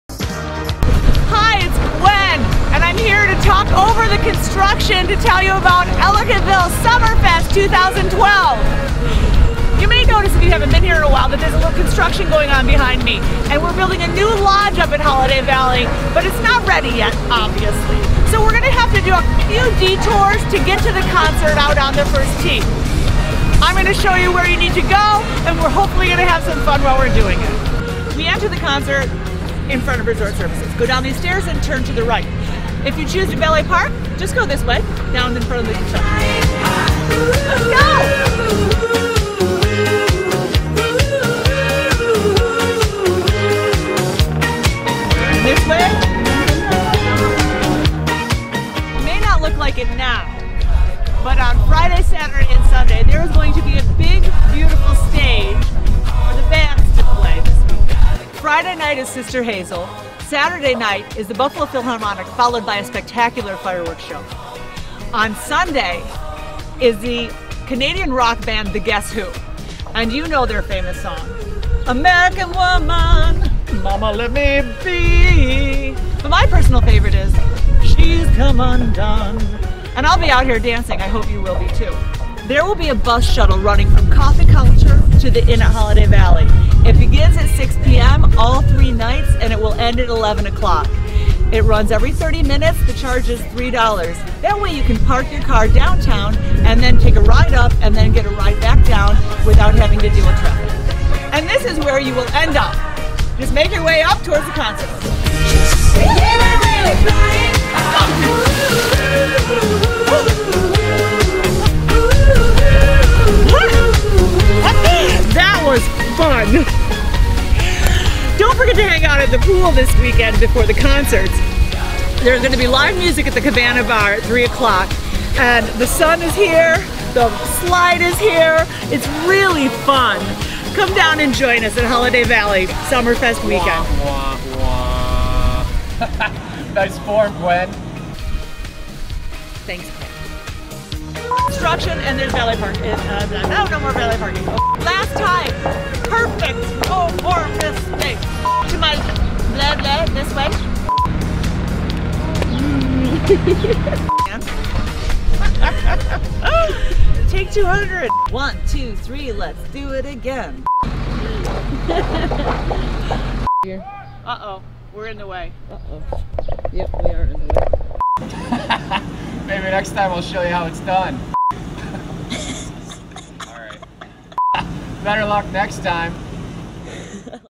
Ellicottville Summer Music Festival!
Gwen Bush gives us the run down of what's going on this weekend at Holiday Valley for the annual Summerfest.  There's still construction going on with the Main Chalet, but it's not going to affect your good time this weekend!